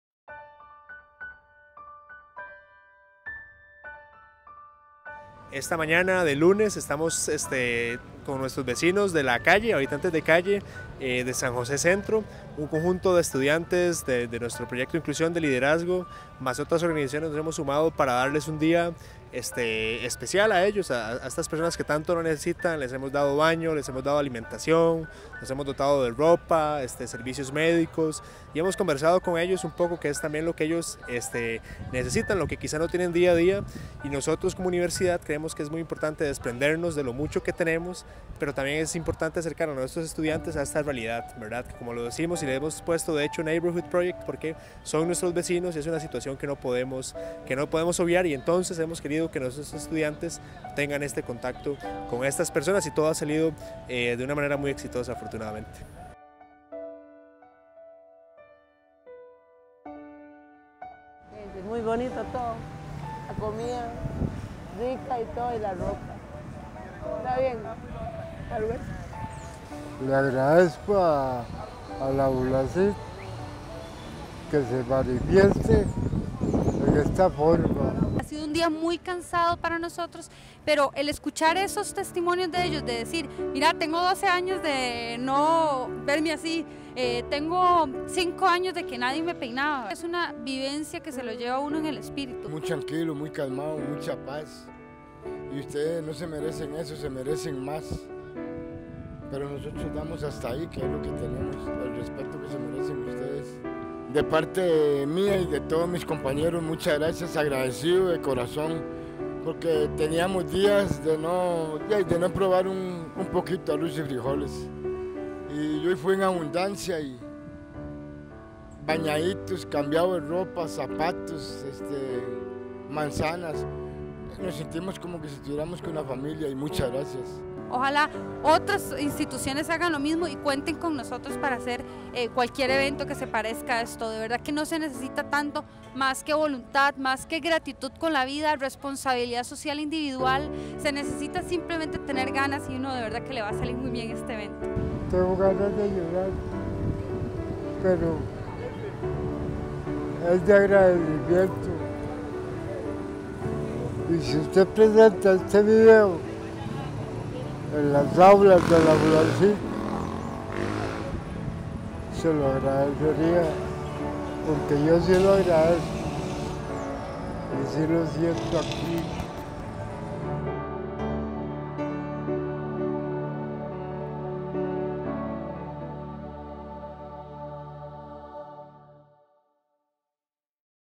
The Neighborhood Project
The Neighborhood Project es el evento en el que, todos juntos, ayudamos a las personas que habitan en las calles de San José.  Durante todo un día, les dimos asistencia para satisfacer sus necesidades de aseo, vestimenta y salud, entre otras.